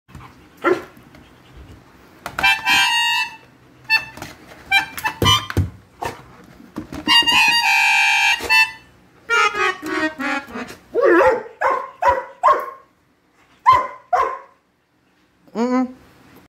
Kylo the Dog plays the Melodica
Our puppy, Kylo a Belgian Malinois plays my blue melodica.